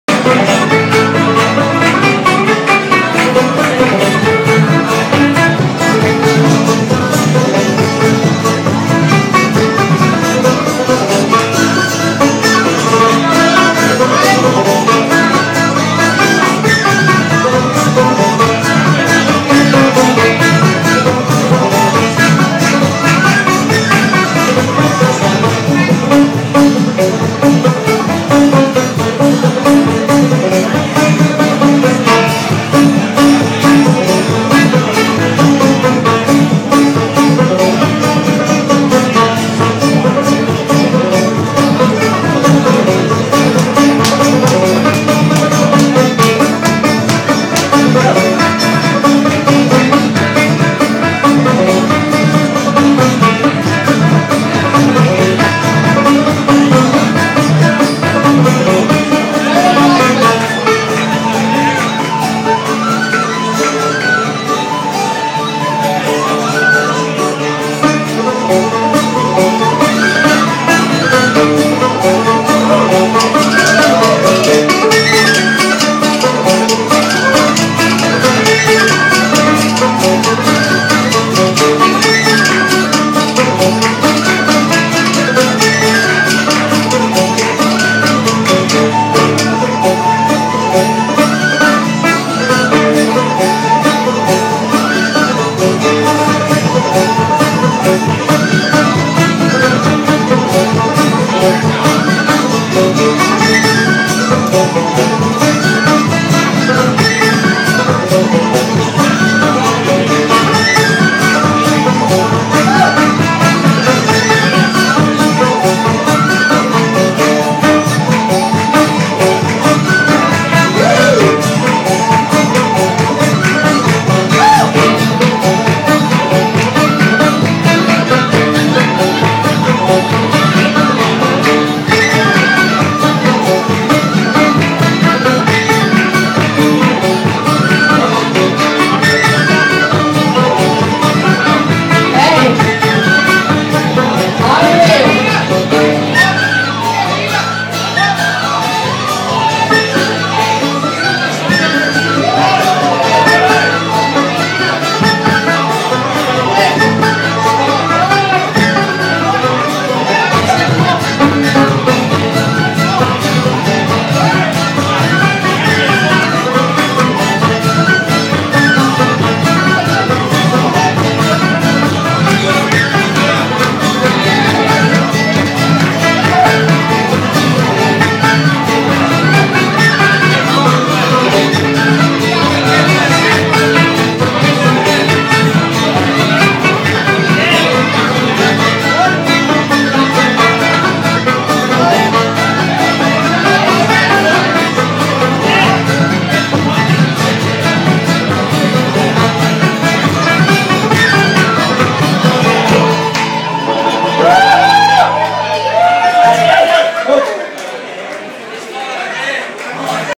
St Pats 2011
Tripping up the stairs (& Friends) at PJ Gallagher's Parramatta - St Patrick's Day 2011